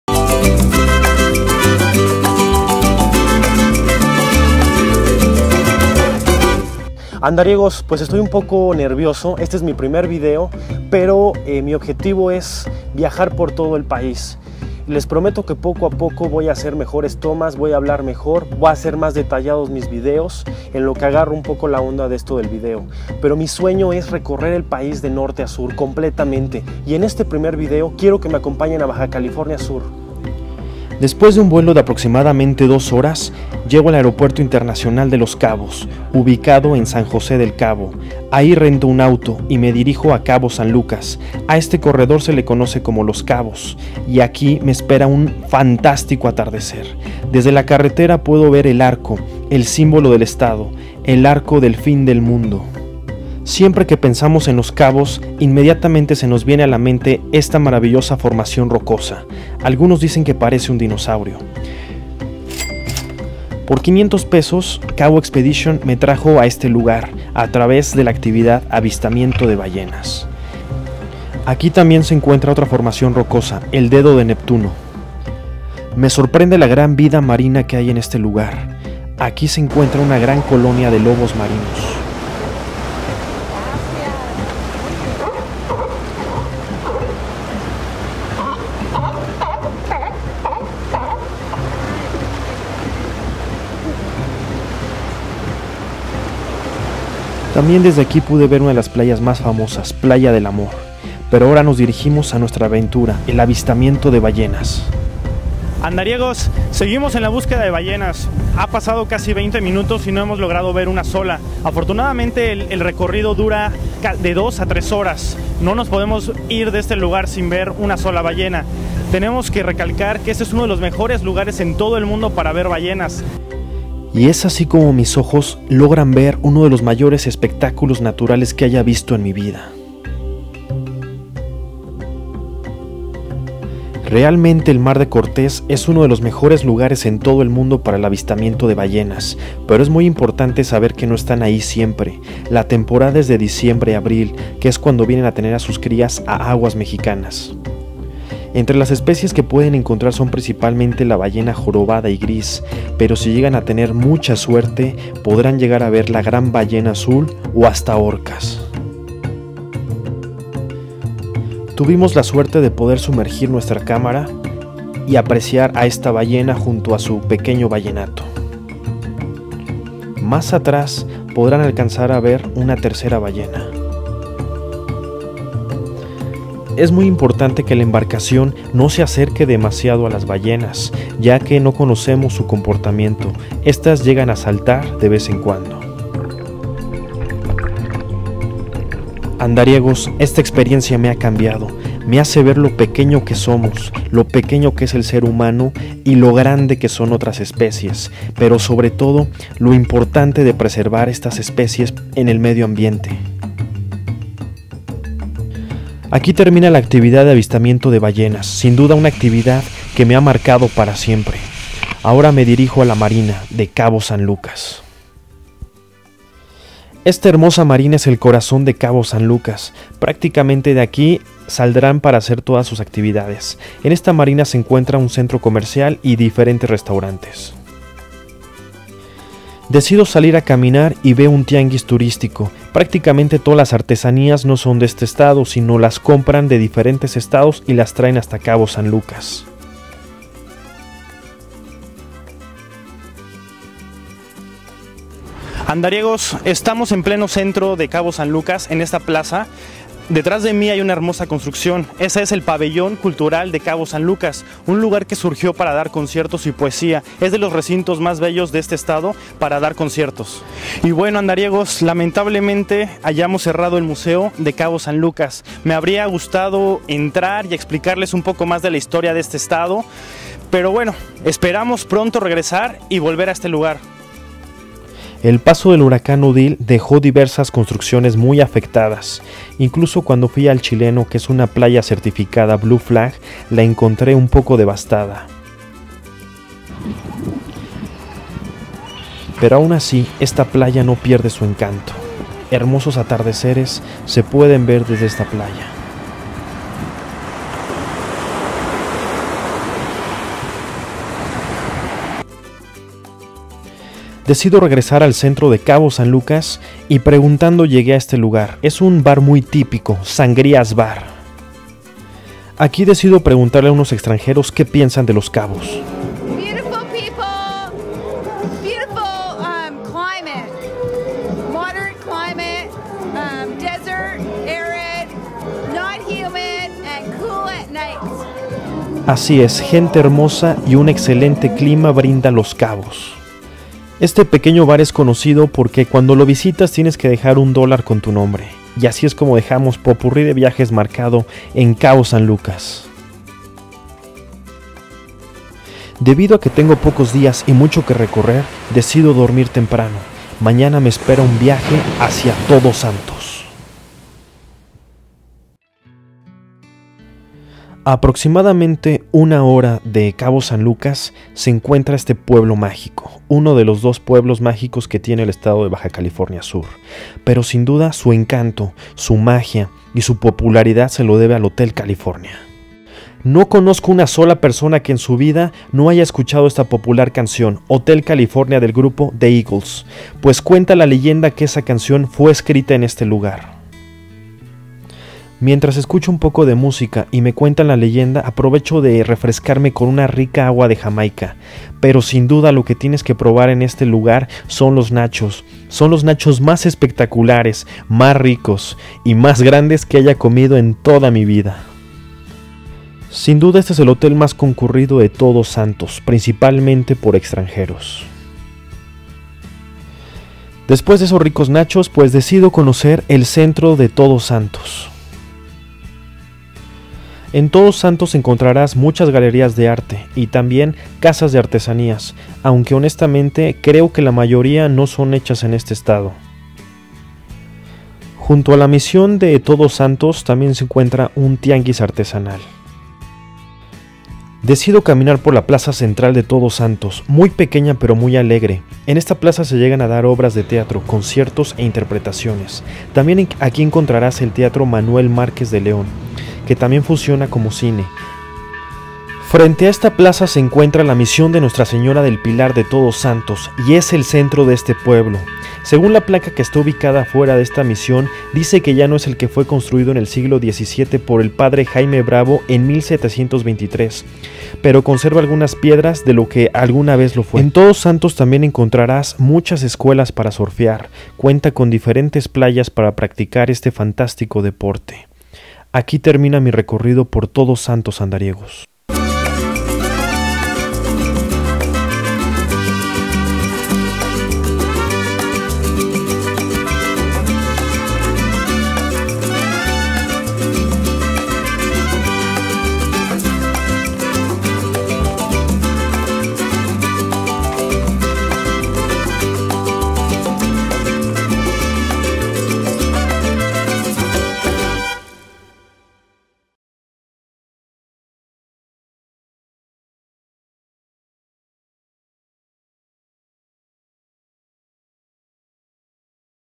De Los Cabos a La Paz video#1/3 Cabo San Lucas y Todos Santos
Tengo pocos dias para recorrer el sur de Baja California Sur, en 5 días muestro opciones a visitar de Los Cabos a La Paz, espero les guste.